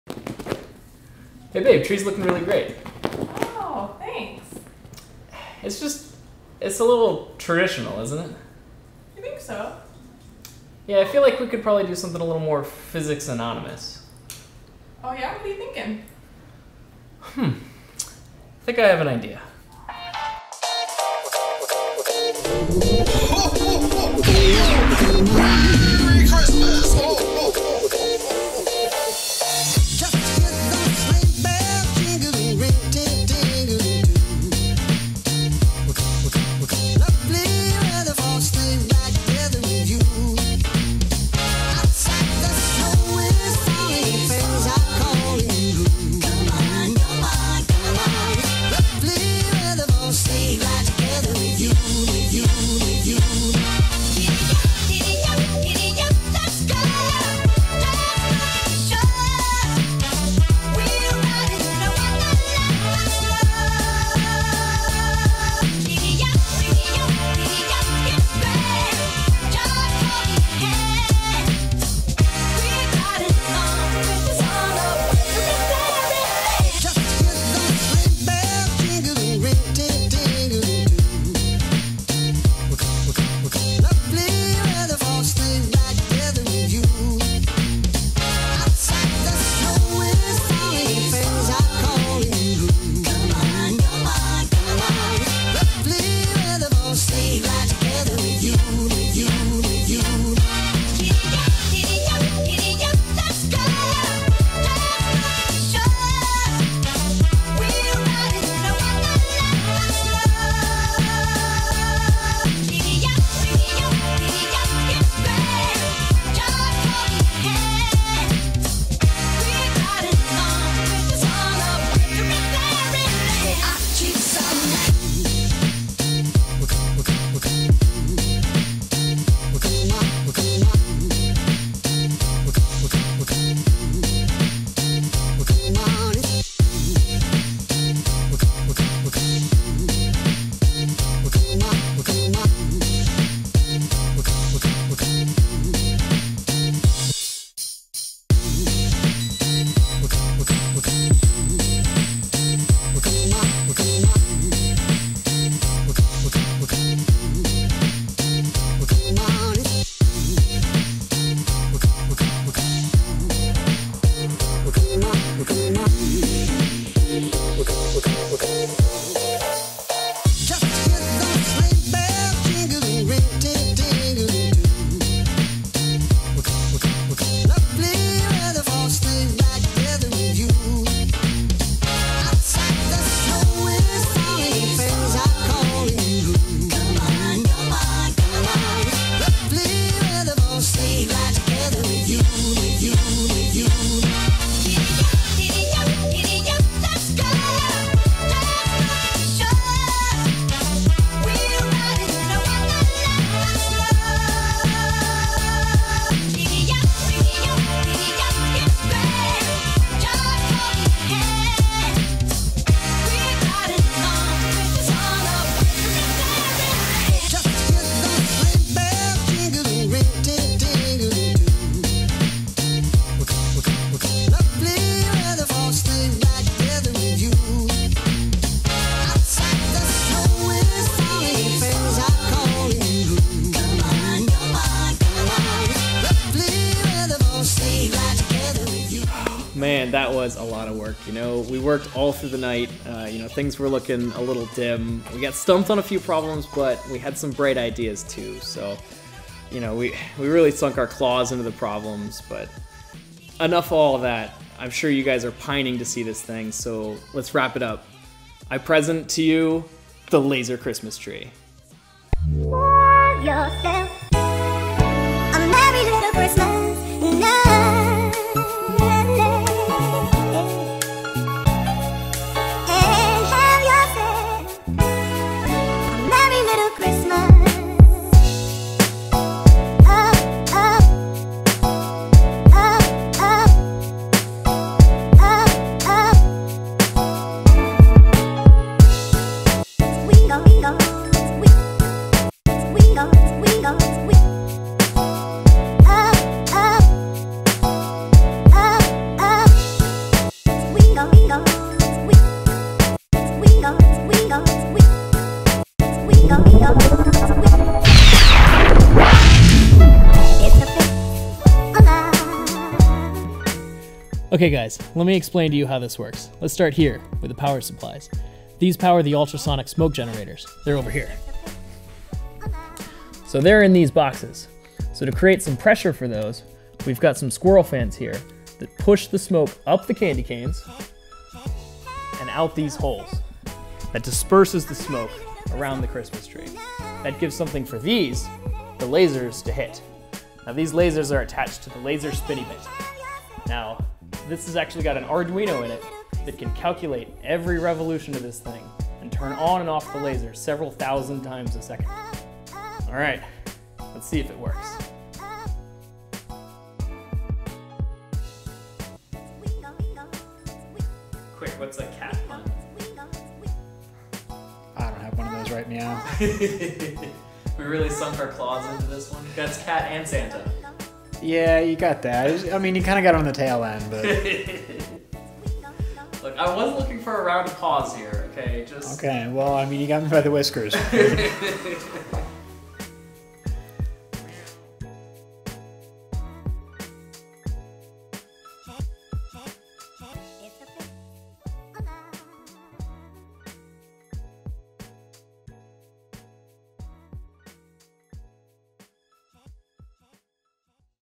S01E15 - Laser Tree
Twas the night before Christmas, when all through the labs,
Not a creature was stirring, except the cat. The cat is always up to something.
The stockings were hung by the oscilloscopes with tape,
In hopes that St Maiman would soon re-awake.

The kitties were nestled all smug in their ways,
While visions of laser-beams danced in the haze.
And Mike in his labcoat and I with my goggles,
Had just settled in for a problem that boggles...

When to my wondering eyes should appear?
But a searing bright light, some fog and an Idea!